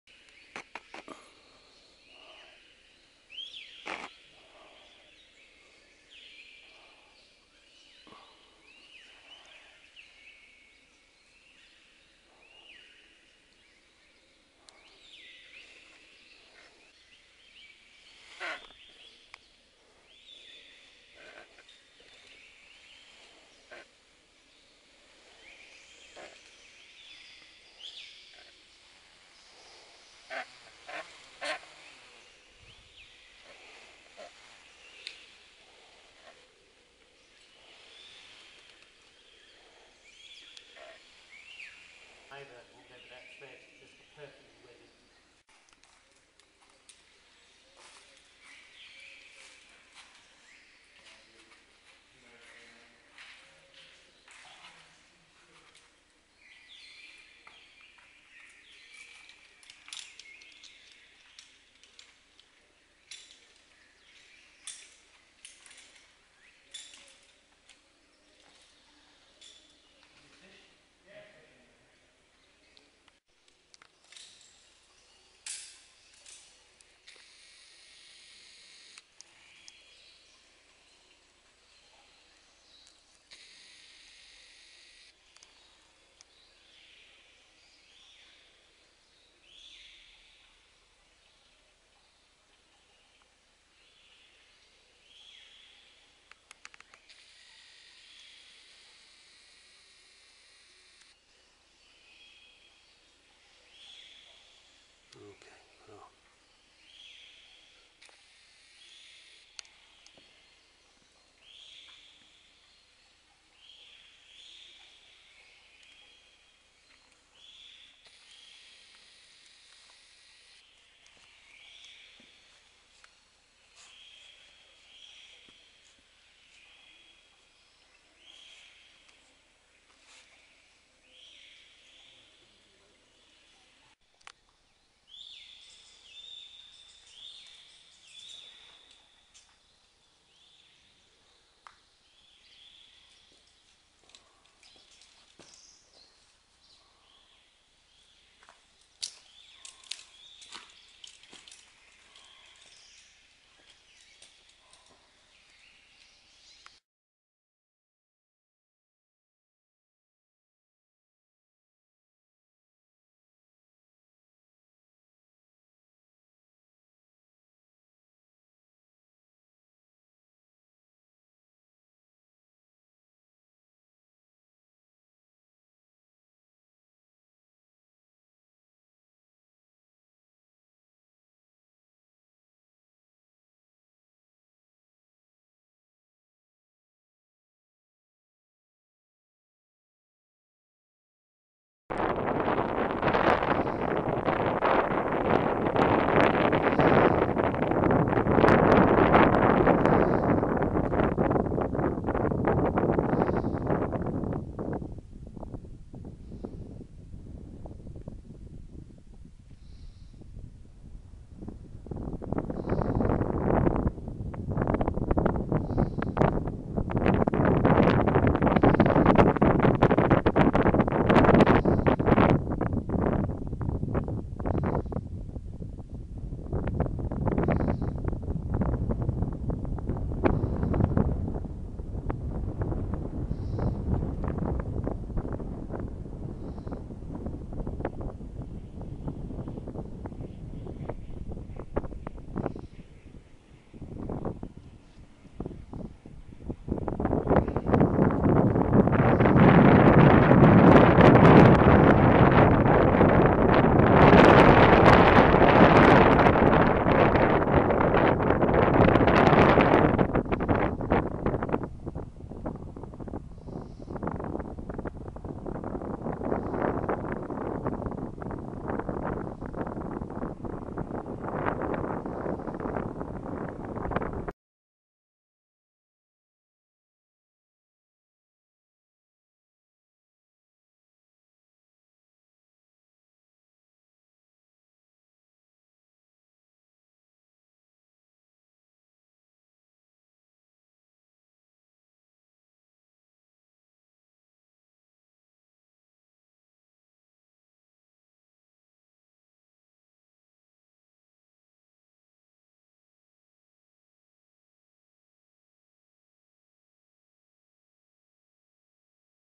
Tawi Attar, Salalah, Sink hole, Oman
Boys Trip to Salalah 2010, Justin, Joe, Sulieman, Rob Gardner.
Dhofar  in Oman is raw and unspoiled by the needs of tourists, it is coming, you can still find the place to explore.
Walking, Nature, Explore, Culture, with Its mountainous area that covers 99300 km2 (38300 sq mi) plenty to do. Dhofar has some great modern history up to 1976,